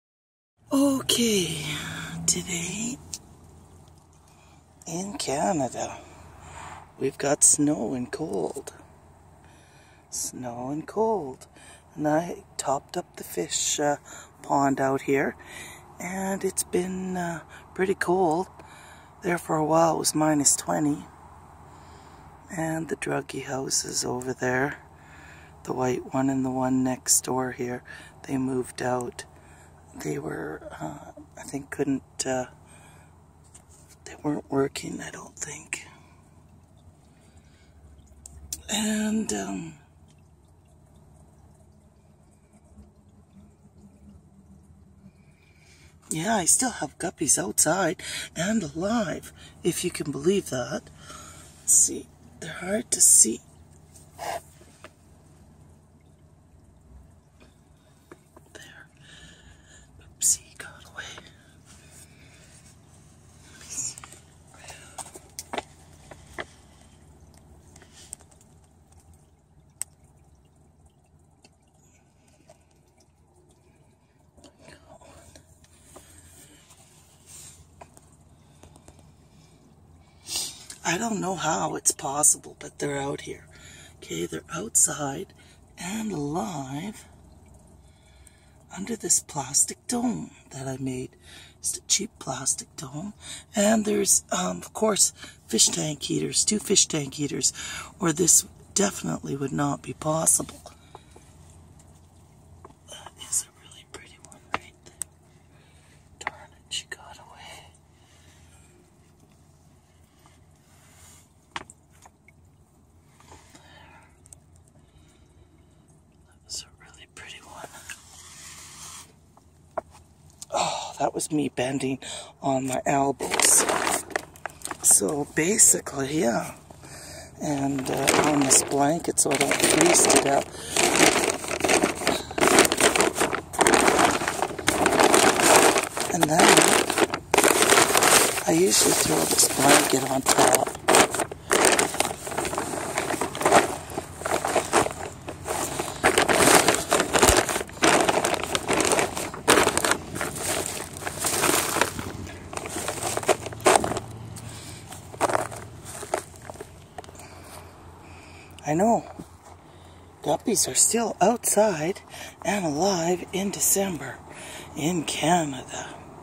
GUPPIES Still Outside in CANADA in DECEMBER !
Hard to believe they are outside still breeding, eating, and Alive this late in the year.  Guppy Experiment continues.  Two fish tank heaters keeping 80 Gallon guppy pond warm enough to make this possible.